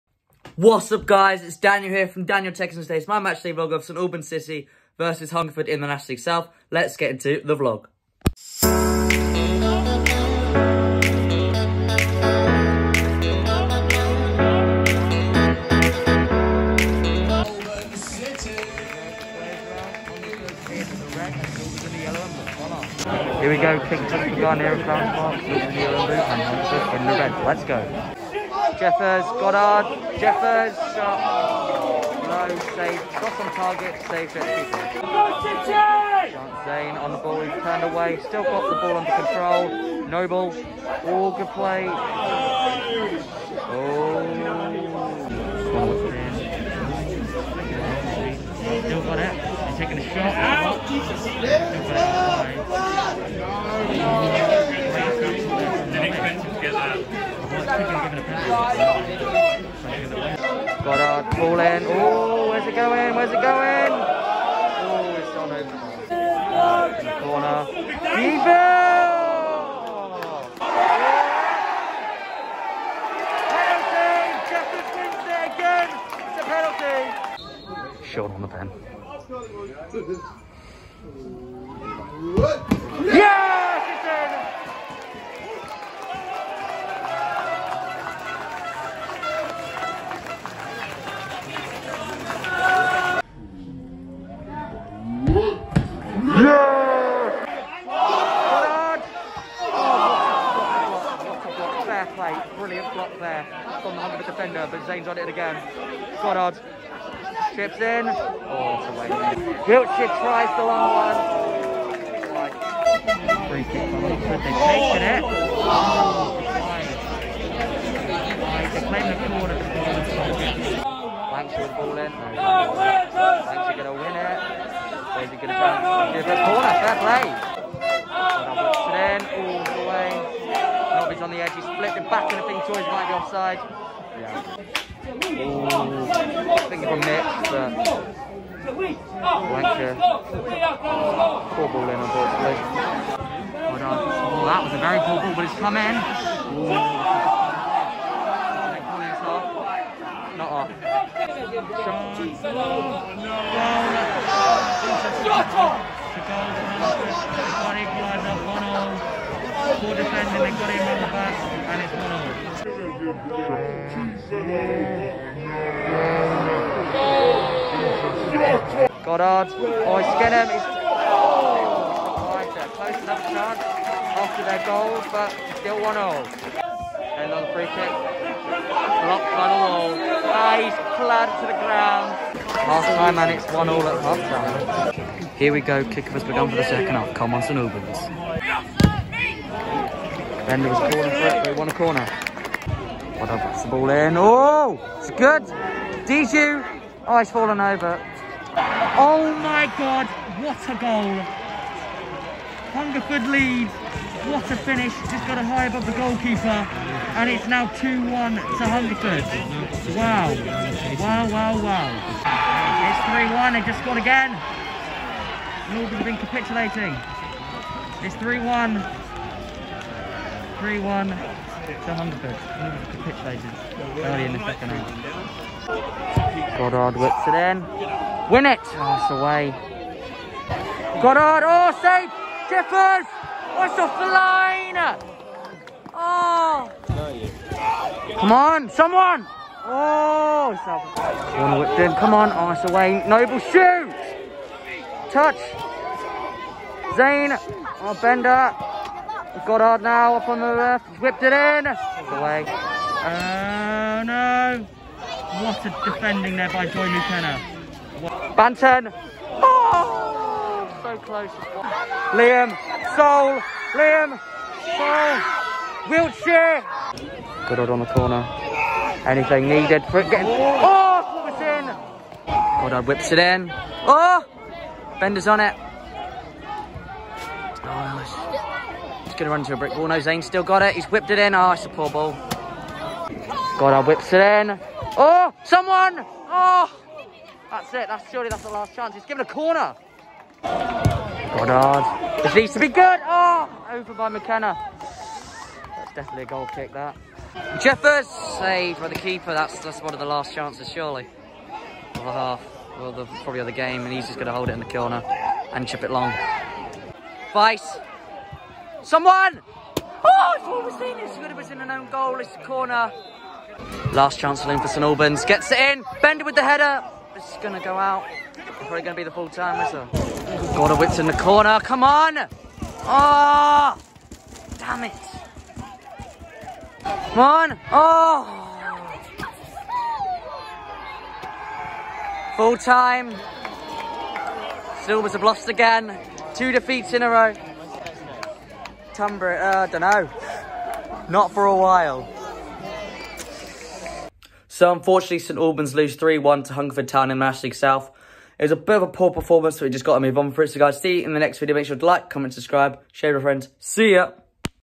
SAINTS MISS BIG CHANCES AS CITY LOSE TWO IN A ROW! ST ALBANS CITY VS HUNGERFORD
Just a very annoying defeat today
Shame for the new goalkeeper to concede three on his debut the defence weren’t switched on today unfortunately 😩